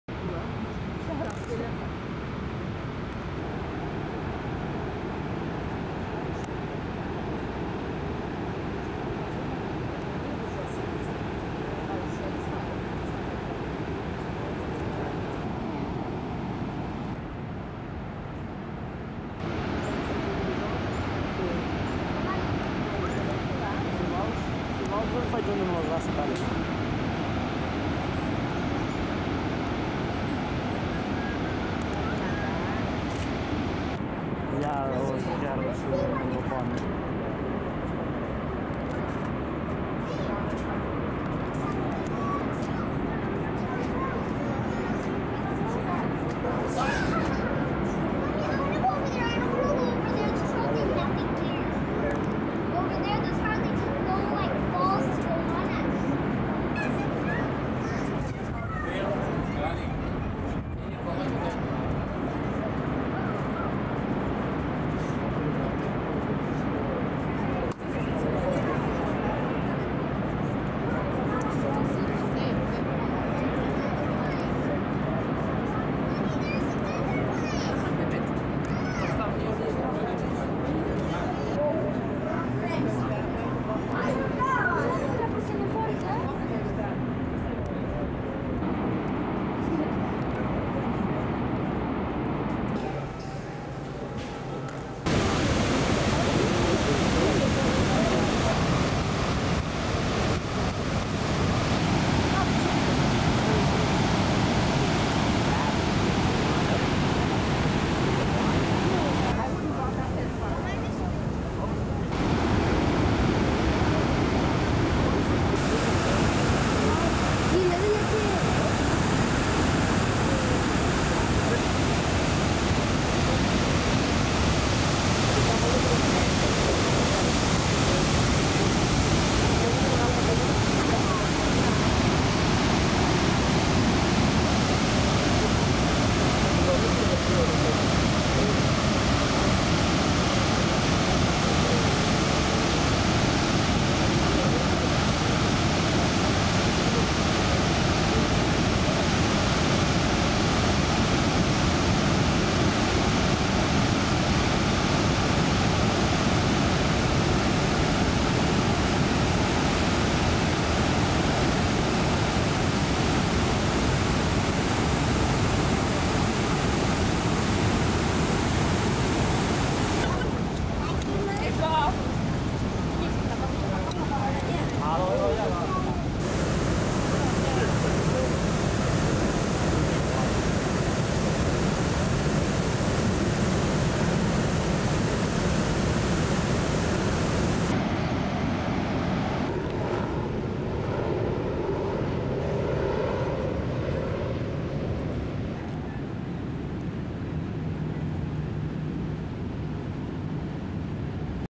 Niàgara. 1995
Imatges de les cataractes del Niàgara al juliol de 1995